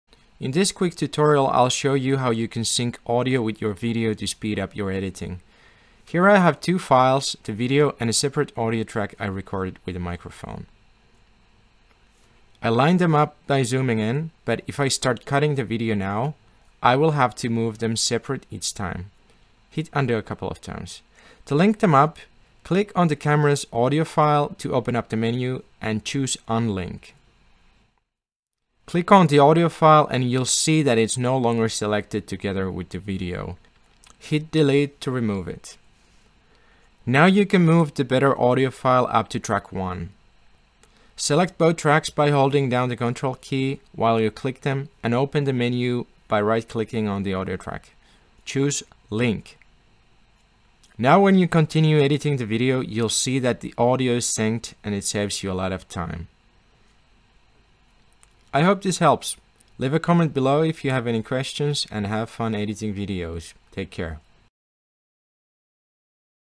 Sync your mic audio with video to speed up editing - HitFilm Express
Hitfilm Express has a great a great feature to help you keep the video and audio in sync. Phones and cameras might not have very good microphones so the sound ends up being thin or echoey. By using a separate mic you get a better sound, but might run into trouble when editing the video to keep everything in sync. Linking them up solves the problem for me.